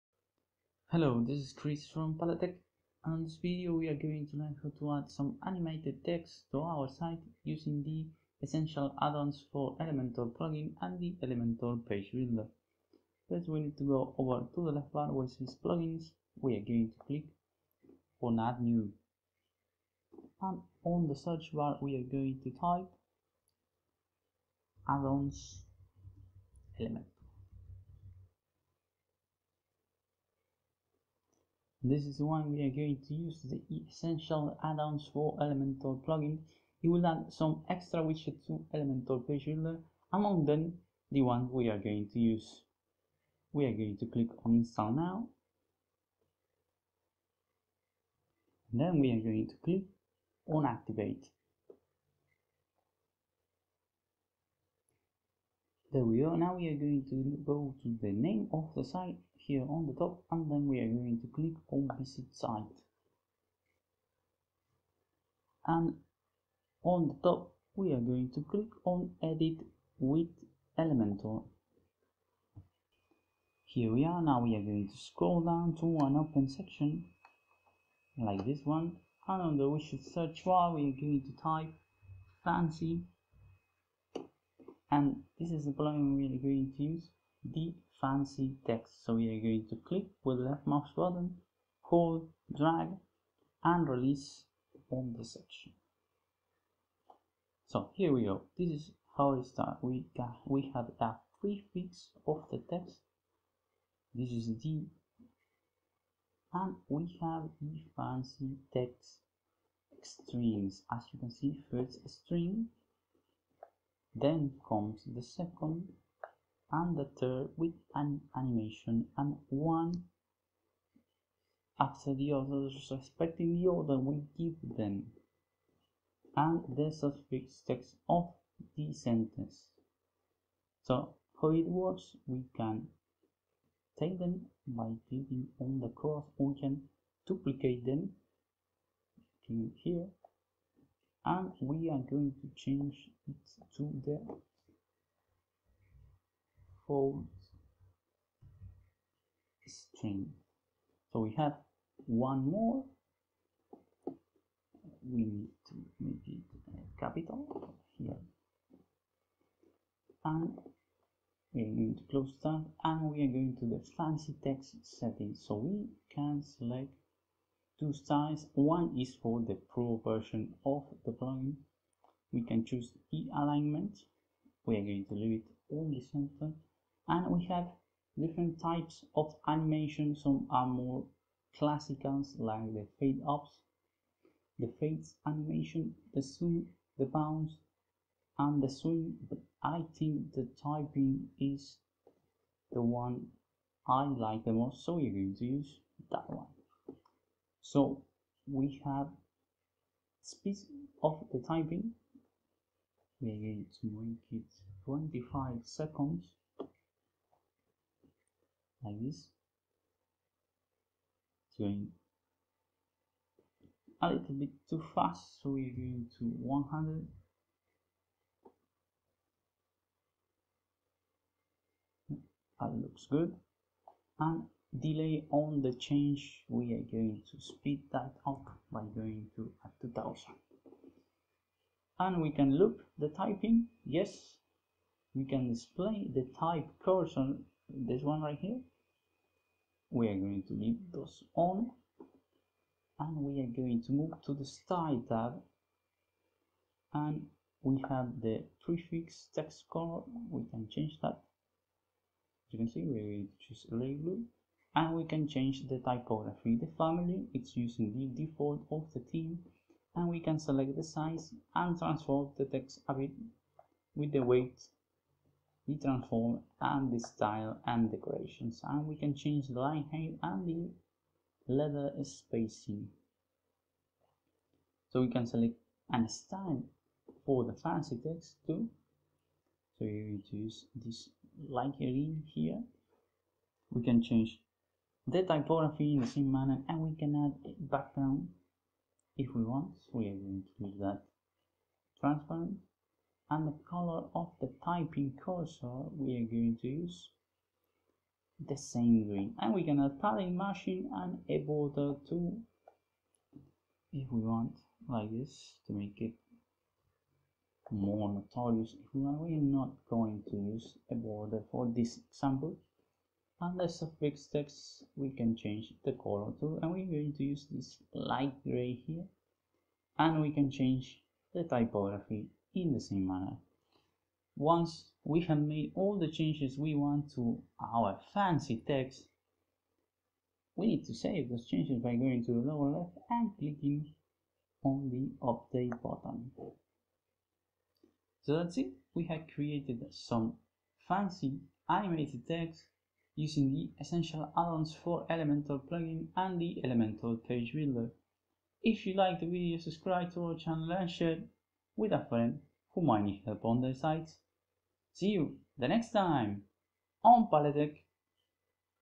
How to add Animated Fancy Text to Wordpress
Learn how you can add Animated Fancy Text to Wordpress with Elementor Page Builder and Essential Addons for Elementor.